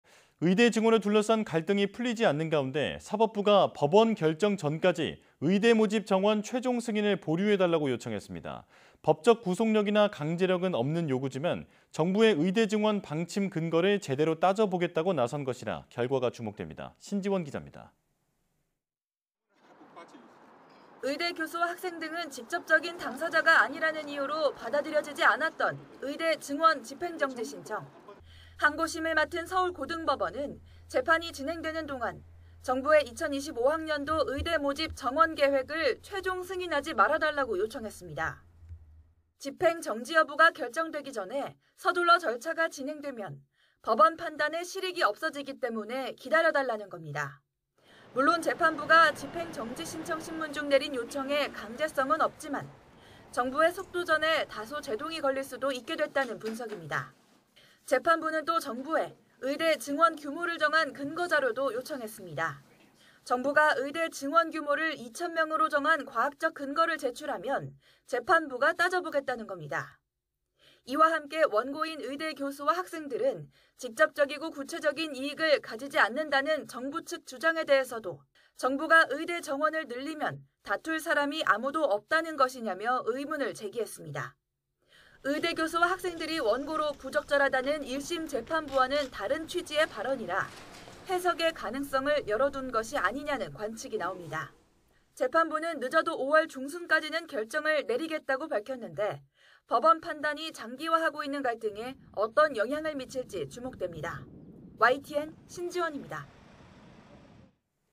법원 "의대 정원 5월 중순까지 승인 보류" 권고...증원 근거도 요청 / YTN 사이언스
[앵커]
의대 증원을 둘러싼 갈등이 풀리지 않는 가운데, 사법부가 법원 결정 전까지 의대 모집 정원 최종 승인을 보류해달라고 요청했습니다.

법적 구속력이나 강제력은 없는 요구지만, 정부의 의대 증원 방침 근거를 제대로 따져보겠다고 나선 것이라 결과가 주목됩니다.

신지원 기자가 보도합니다.

[기자]
의대 교수와 학생 등은 직접적인 당사자가 아니라는 이유로 받아들여지지 않았던 '의대 증원 집행정지 신청'.

항고심을 맡은 서울고등법원은 재판이 진행되는 동안, 2025학년도 의대 모집 정원 계획을 최종 승인하지 말아 달라고 정부에 요청했습니다.

집행정지 여부가 결정되기 전에 서둘러 절차가 진행되면 법원 판단의 실익이 없어지기 때문에 기다려달라는 겁니다.

물론, 재판부가 집행정지 신청 심문 중 내린 요청에 강제성은 없지만, 정부의 속도전에 다소 제동이 걸릴 수도 있게 됐단 분석입니다.

재판부는 또 정부에 의대 증원 규모를 정한 근거 자료도 요청했습니다.

정부가 의대 증원 규모를 2천 명으로 정한 과학적 근거를 제출하면, 재판부가 따져보겠다는 겁니다.

이와 함께, 원고인 의대 교수와 학생들은 직접적이고 구체...